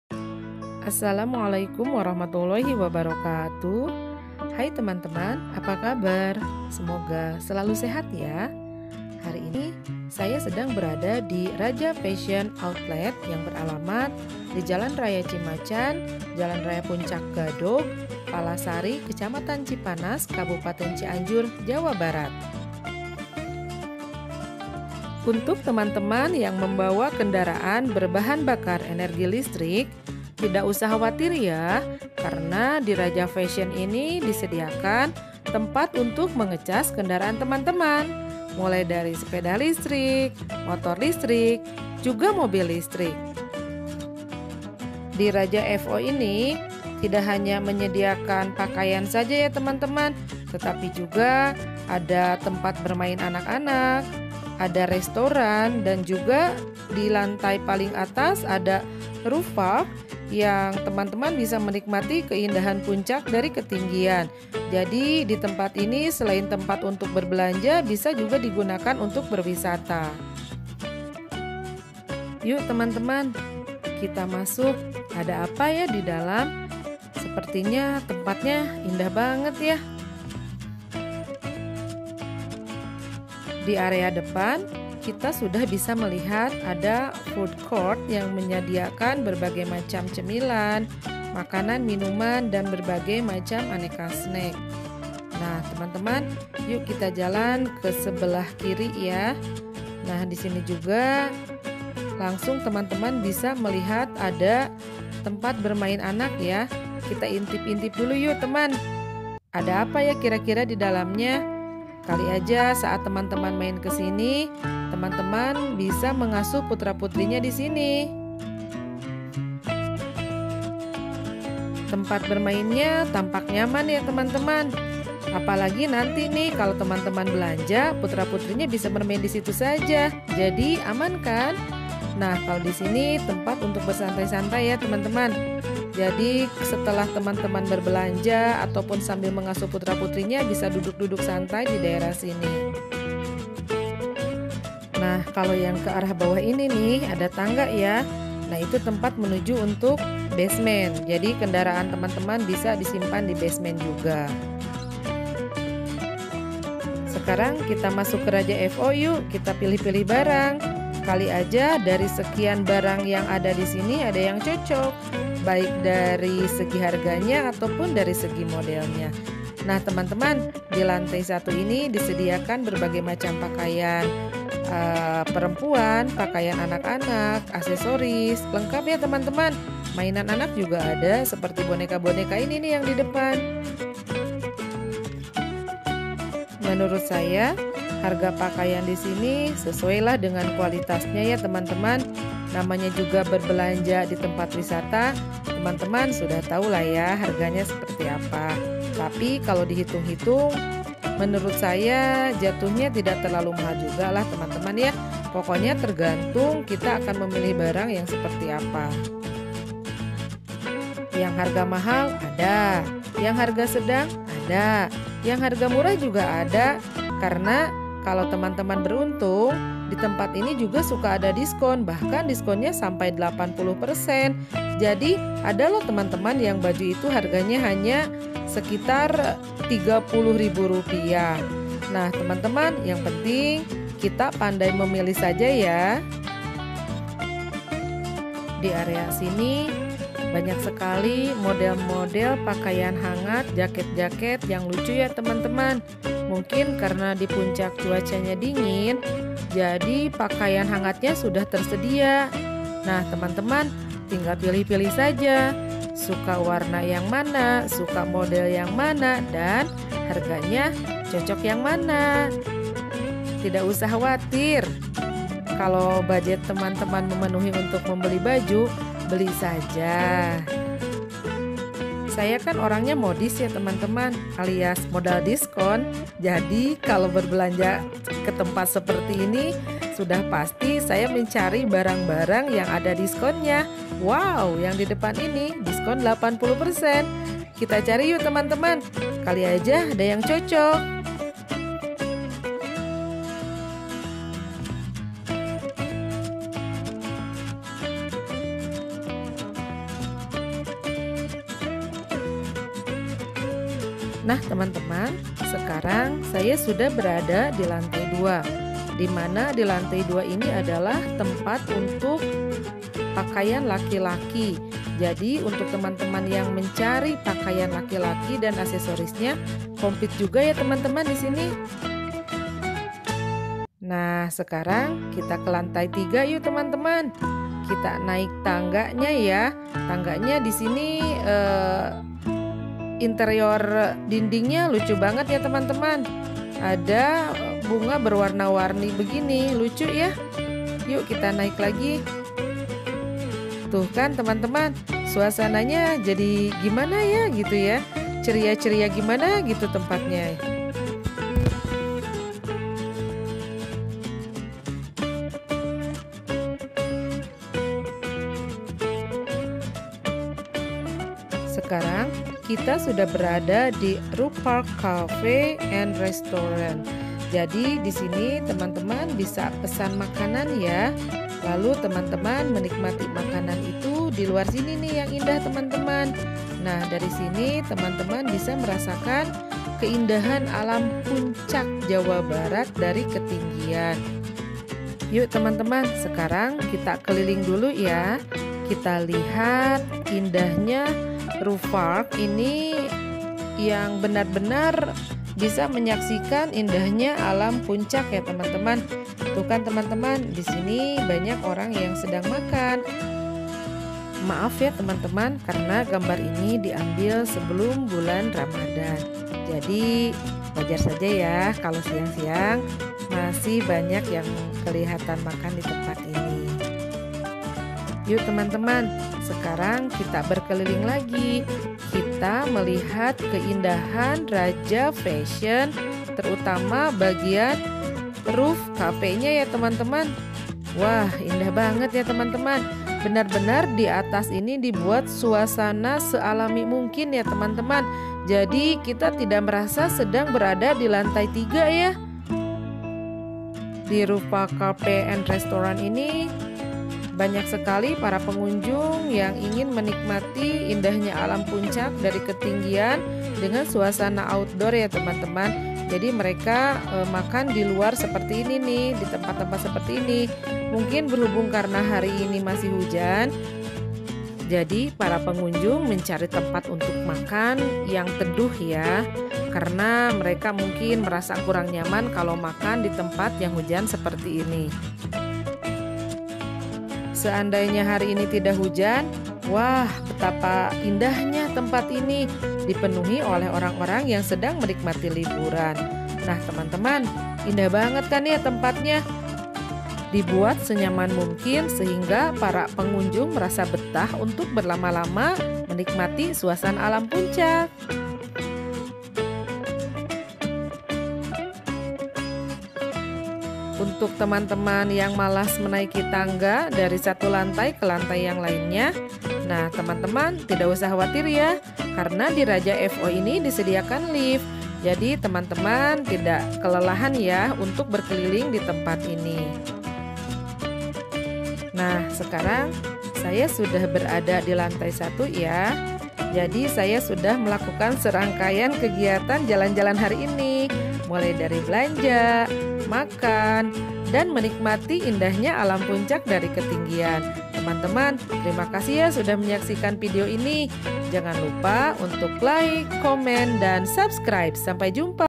Raja FO dan RoofPark Puncak, Seru dan Menyenangkan
Raja Fashion Outlet beralamat di Jalan Cimacan Puncak Jawa Barat.
Disini adalah tempat belanja pakaian juga aksesorisnya untuk pria dan wanita juga ada pakaian anak. Tempatnya bagus karena ada RoofPark nya, ada restaurannya juga pastinya ada tempat bermain anak.
Selamat berlibur.